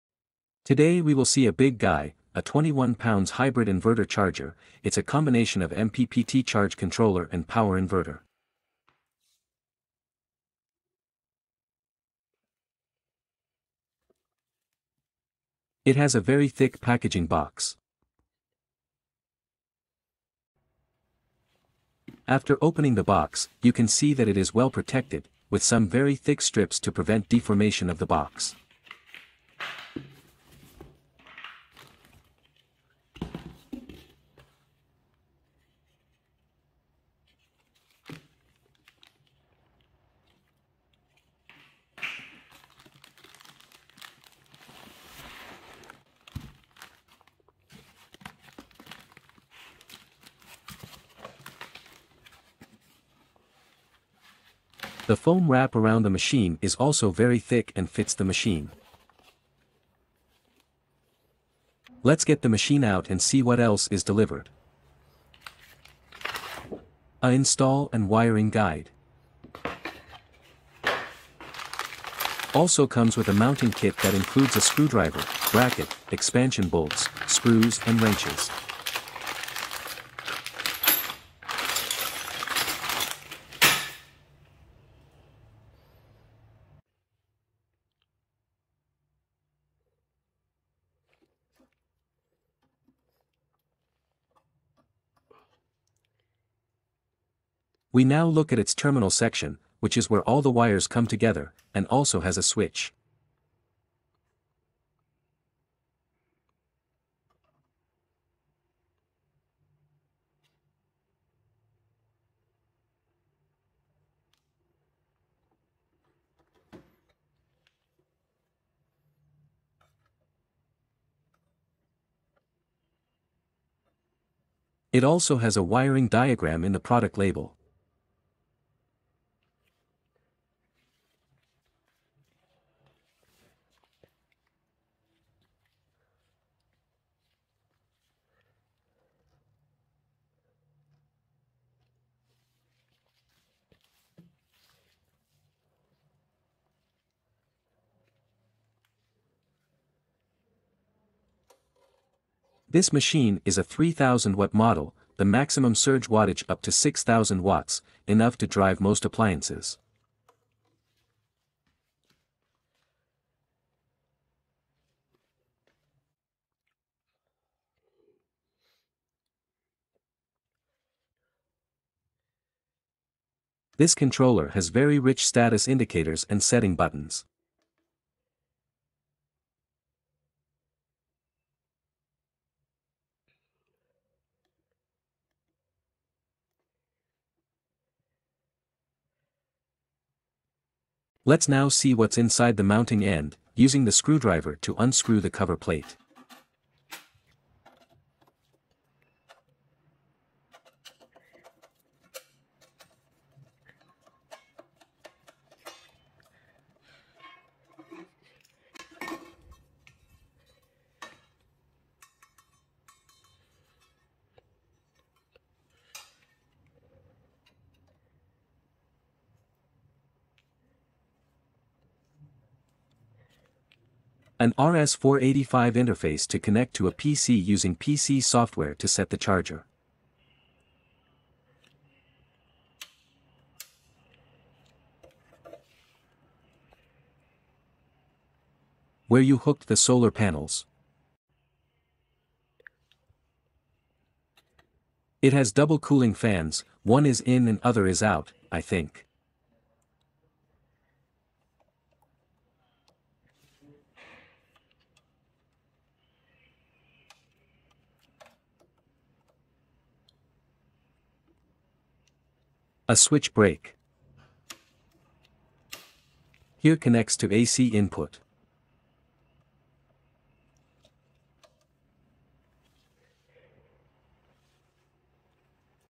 EPever Inverter Charger 3000W Unboxing, Hybrid Solar Charge Controller Inverter
The EPever UPower Inverter Charger is a new type of the hybrid inverter/ MPPT charger combining with solar & utility charging and AC output, which adopts a multi-core processor design and advanced MPPT control algorithm, and has the features of high response speed, high reliability, and high industrialization standard. It offers four charging modes to Solar priority, Utility priority, Solar and Utility & Solar; two output modes for Battery and Utility, meeting the various application demands.

The up-to-date optimized MPPT tracking technology is adopted for the PV charging modules. It can quickly track the maximum power point of the PV array in any environment and acquire the maximum energy of solar panel in real-time;

The advanced control algorithm is adopted for AC-DC charging modules that realize fully digitalized double closed-loop control for voltage and current, with high control precision, small volume. The input range of AC voltage is wide, the output DC charging voltage/current is continuously adjustable in a certain range, and the complete input/output protection functions can offer stable and reliable charging and protection for the battery.

The DC-AC inverter modules are based on full digital and intelligent design. It adopts the advanced SPWM technology, outputs the pure sine wave and converts 24/48VDC to 220/230VAC suitable for AC loads of household appliances, electric tools, commercial units, electronic audio, and video devices etc.

The product adopts 4.2 inch LCD display design, which real-time displays the operational data and running state of the system. The comprehensive electronic protection function guarantees a safer and more stable operation of the solar power system.